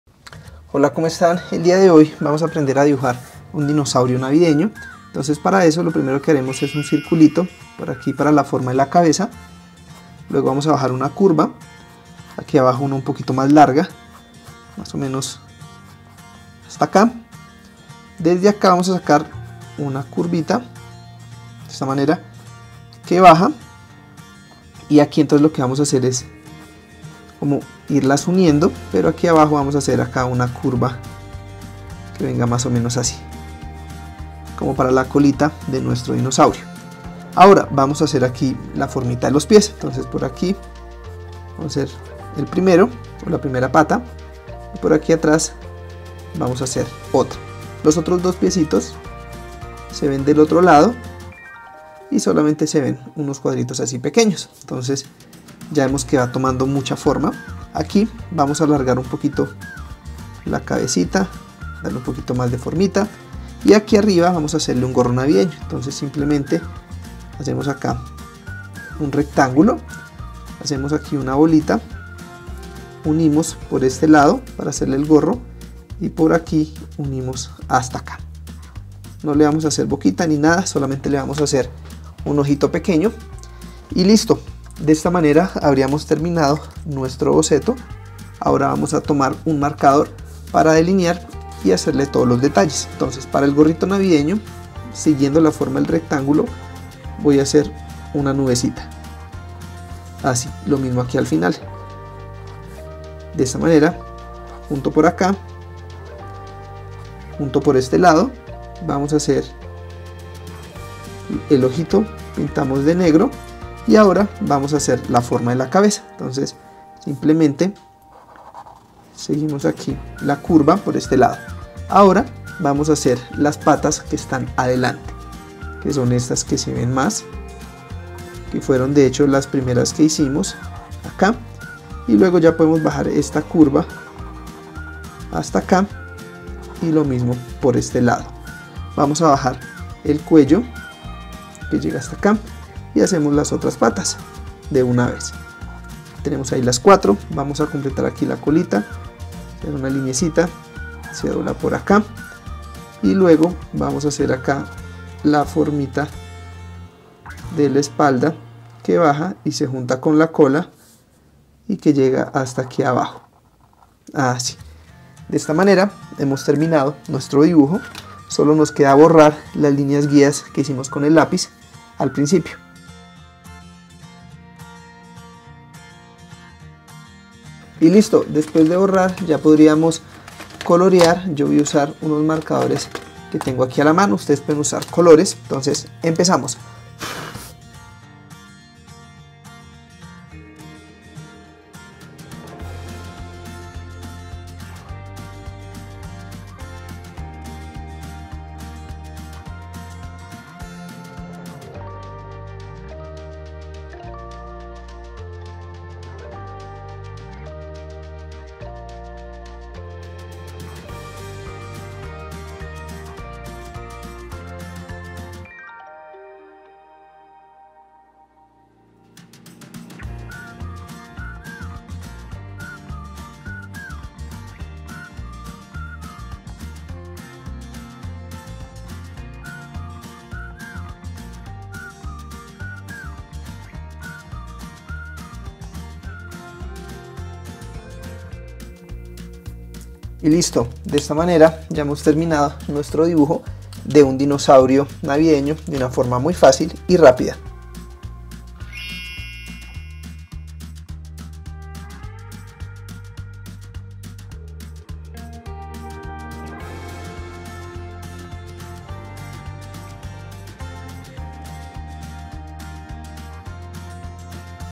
cómo dibujar un dinosaurio navideño | Dibujos de Navidad
Aprende cómo dibujar Dinosaurio Navideño paso a paso y de la forma más fácil. Tutoriales de dibujo de Navidad para que aprendas a dibujar dibujos fáciles.